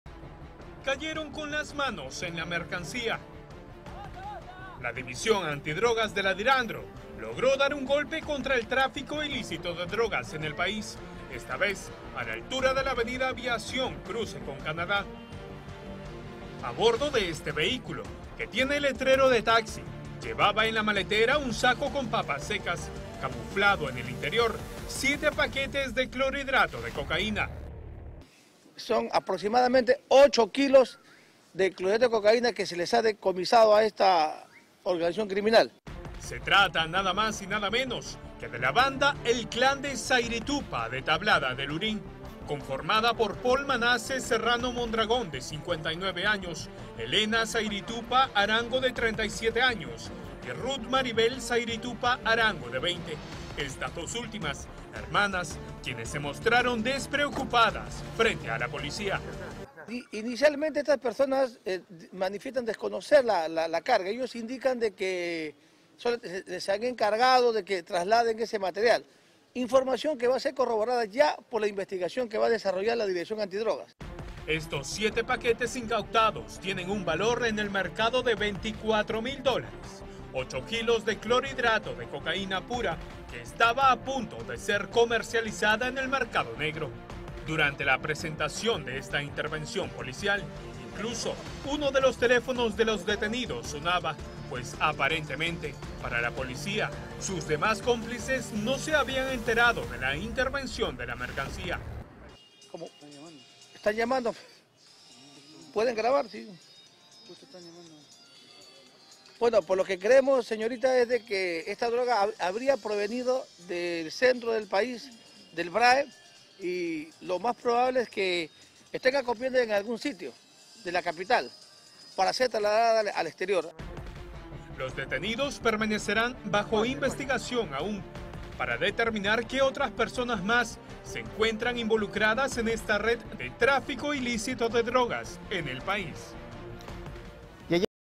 Intervienen taxi y hallan 8 kilos de droga camuflada en sacos de papa seca
Tras días de seguimiento, se intervino a Paul Manases Serrano Mondragón (59), Helena Sairitupac Arango (37) y Ruth Maribel Sairitupac Arango (20), miembros de la banda &#39;El clan Sairitupac de Tabalada de Lurín&#39;, con 8 kilos de clorhidrato de cocaína pura, en su poder.

Estos transportaban la droga a bordo de un taxi, camuflada en sacos de papa seca. “Inicialmente estas personas manifiestan desconocer la carga”, informó un agente de la PNP.

Según investigación, la mercancía tendría un valor aproximado de 24 mil dólares, y pretendía ser comercializada al exterior del país.

“Esta droga habría provenido del centro del país, del VRAEM, y lo más probable es que tenga convenio en algún sitio de la capital, para ser trasladada al exterior”, acotó un portavoz de la PNP.

Según información brindada por los agentes policiales, los detenidos permanecerían bajo investigación, mientras se determina la procedencia de los estupefacientes, y si hubiera otras personas involucradas en esta red de tráfico ilícito de drogas, al interior del país